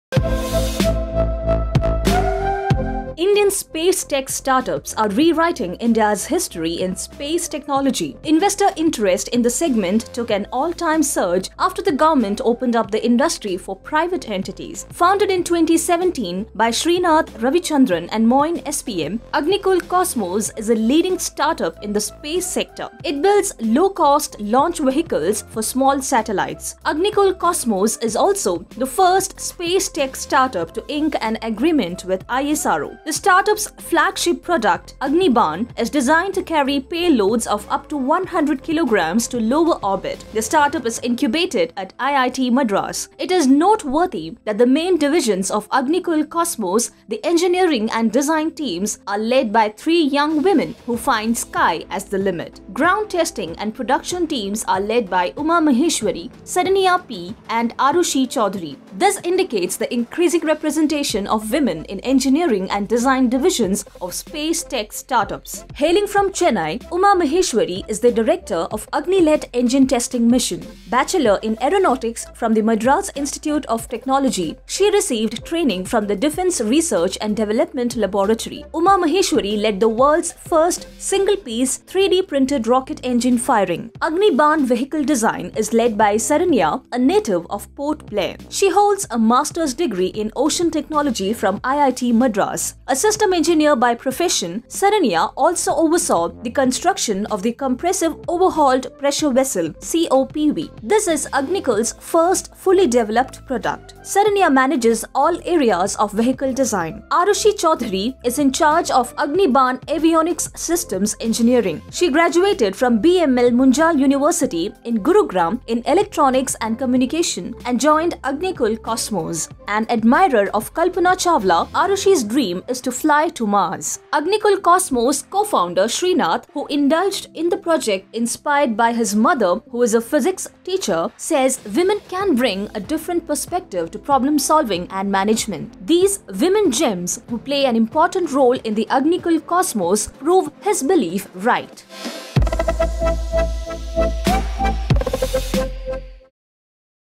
Meet the women who steer the initiatives behind Agnikul, India's space tech startup
Indian space tech startups are rewriting India's history in space technology. Investor interest in the segment took an all-time surge after the government opened up the industry for private entities. Founded in 2017 by Srinath Ravichandran and Moin SPM, Agnikul Cosmos is a leading startup in the space sector. It builds low-cost launch vehicles for small satellites. Agnikul Cosmos is also the first space tech startup to ink an agreement with ISRO. The startup's flagship product 'Agnibaan' is designed to carry payloads of up to 100 kilograms to lower orbit. The startup is incubated at IIT Madras.